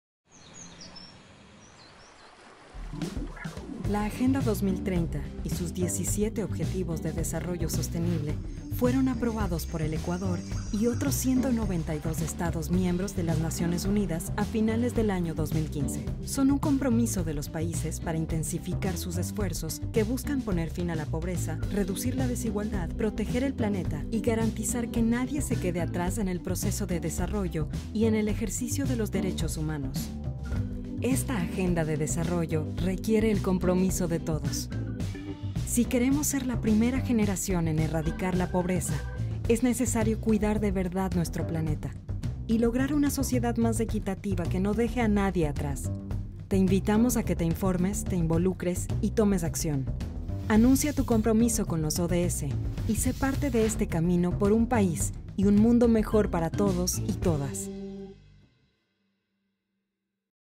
¿Qué se requiere para asegurar un Ecuador sostenible, equitativo, próspero y en paz?
En 2015, Ecuador se comprometió en la ONU, con la Agenda 2030 y los 17 Objetivos de Desarrollo Sostenible para intensificar sus esfuerzos en poner fin a la pobreza en todas sus formas, reducir la desigualdad y proteger el planeta, garantizando que nadie se quede atrás en el proceso de desarrollo ni en el ejercicio de los derechos humanos.